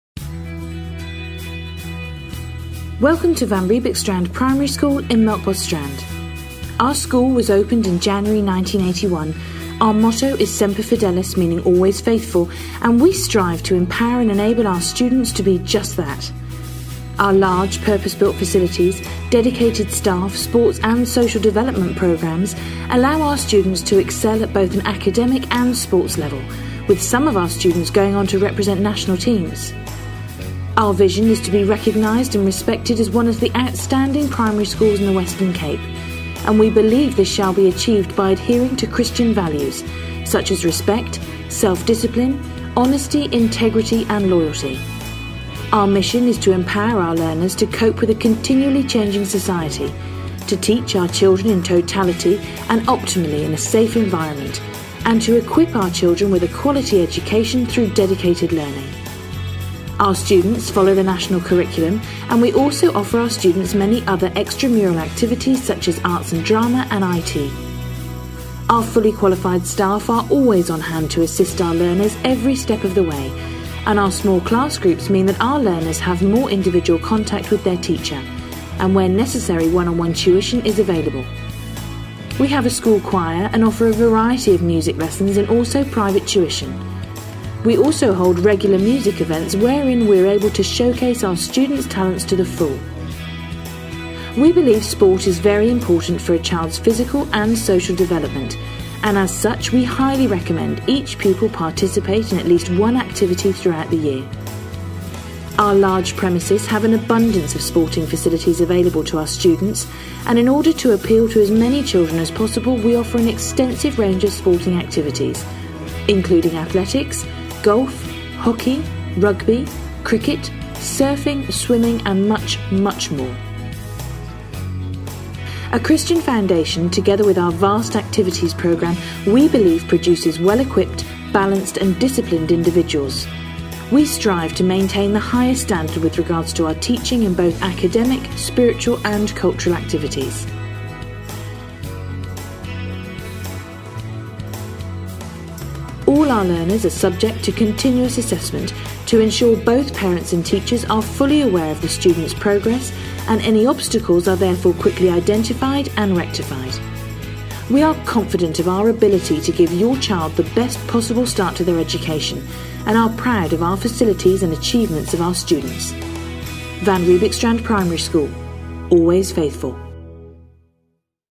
Van Riebeeckstrand Skool.flv
Welcome to Van Riebeeckstrand  Primary School in Melkbos
Our school was opened in January 1981, our Motto is Semper Fidelis meaning Always Faithful and we strive to empower and enable our students to be just that.
Our large purpose built facilities, dedicated staff, sports and social development programmes allow our students to excel at both an academic and sports level with some of our students going on to represent national teams.